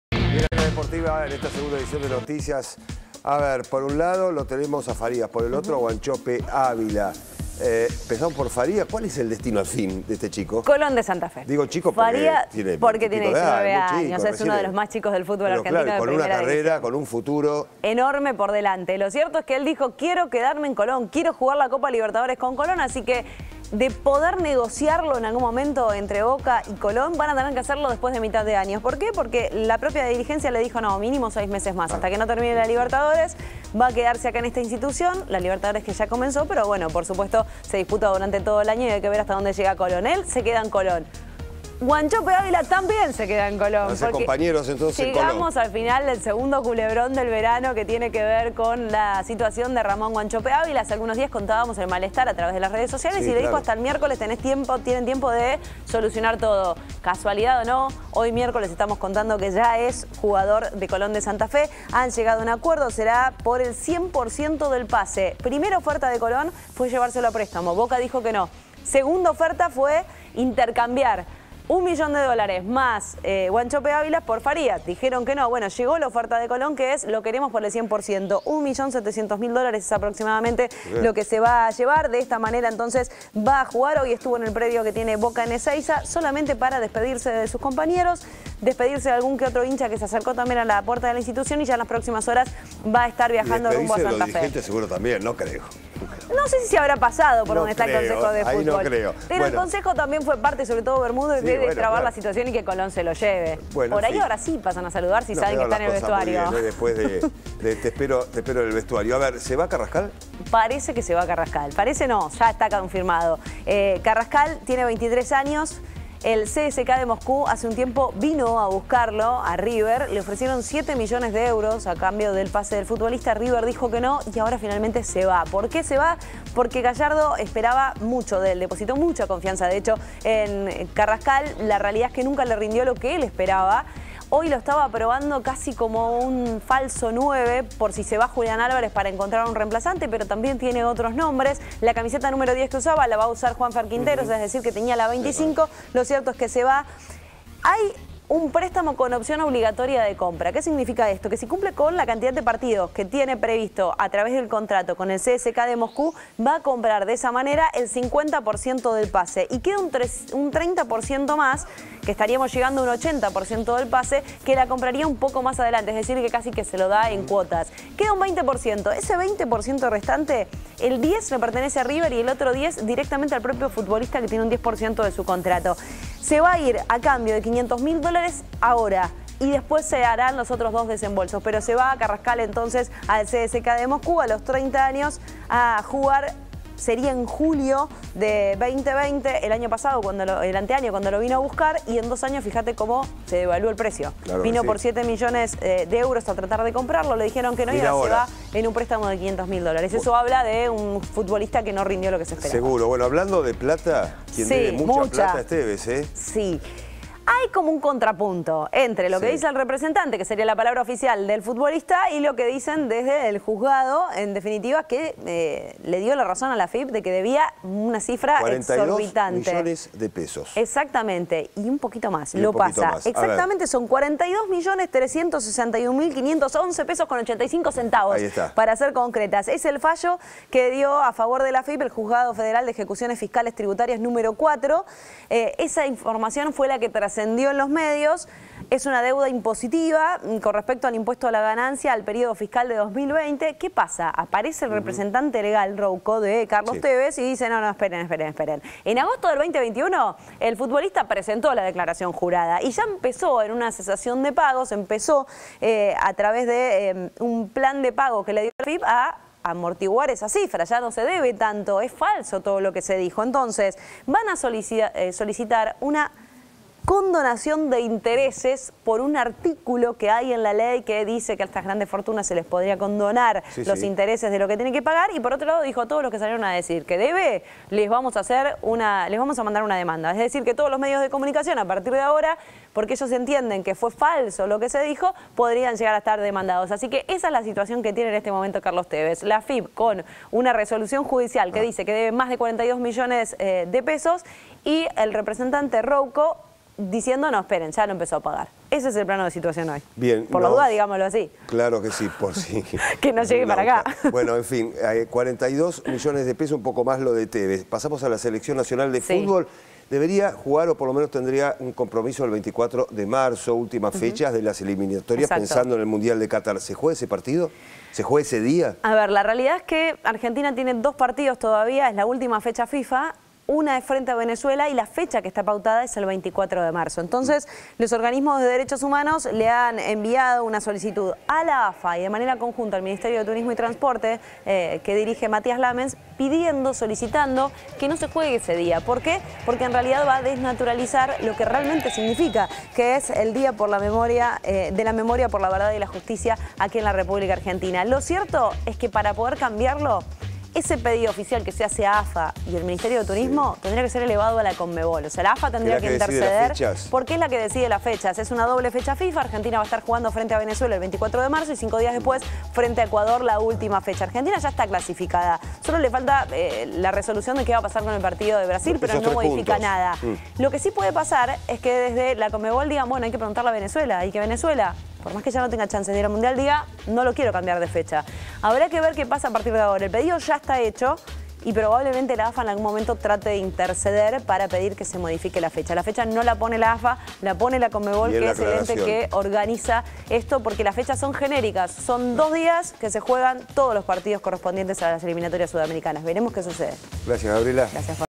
Wanchope a Colón; Carrascal, ¿a Rusia? y Tévez a la AFIP
Gabriela Previtera presenta la agenda del fútbol argentino. Al final, se concretó el pase del delantero al sabalero pero no el de Farías. El colombiano de River se iría a Europa. El Apache deberá pagar $ 42 millones de deuda impositiva. Organismos de derechos humanos pidieron que la Selección no juegue el 24 de marzo.